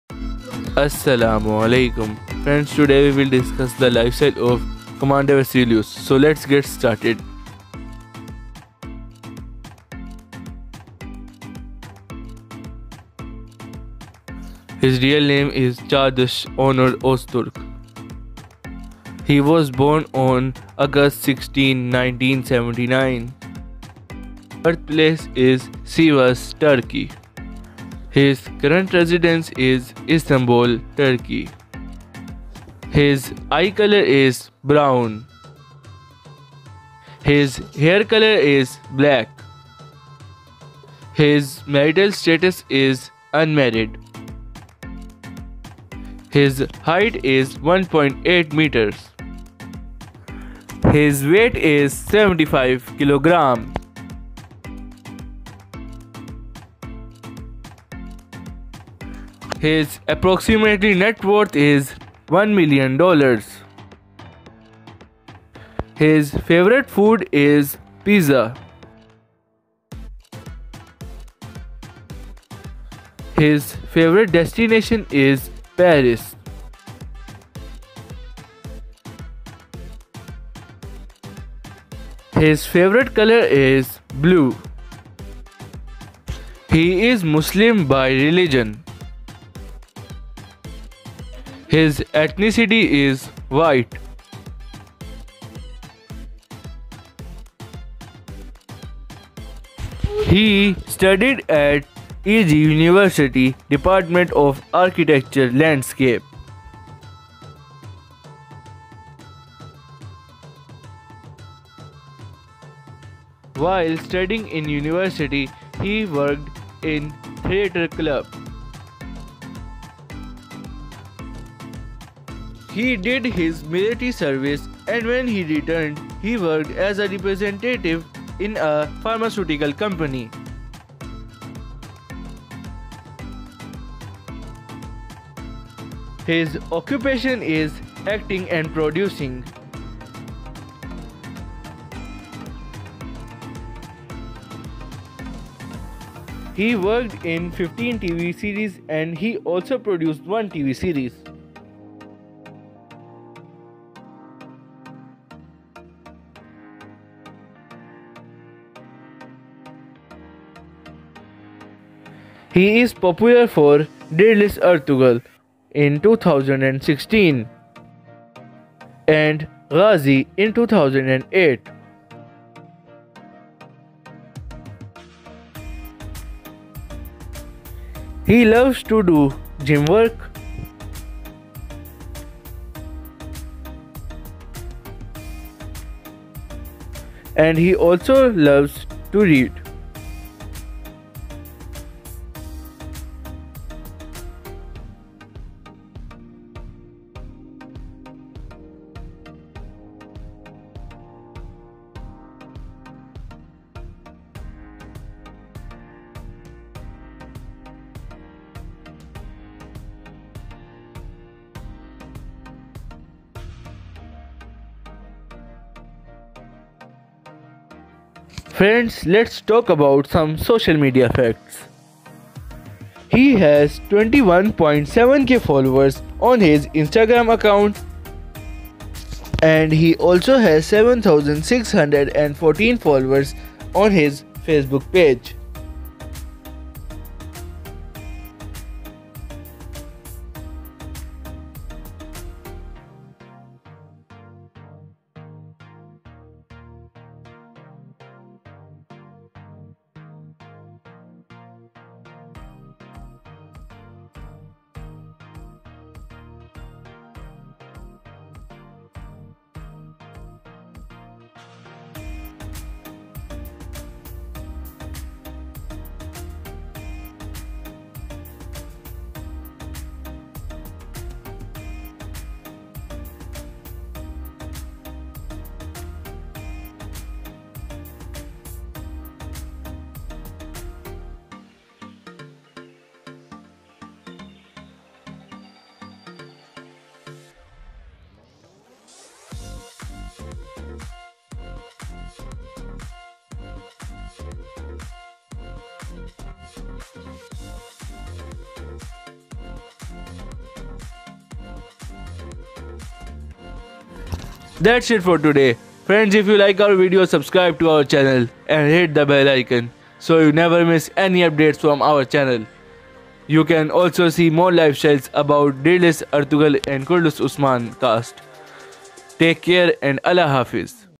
Vasilius lifestyle|Vasilius real name|ShaheerTv
Assalam o Alaikum friends today we are sharing lifestyle of Cagdas onur Ozturk also known as Commander Vasilius in Dirilis Ertugrul. In this video you will see this real family , net worth , career and much more . Hope you enjoy this video.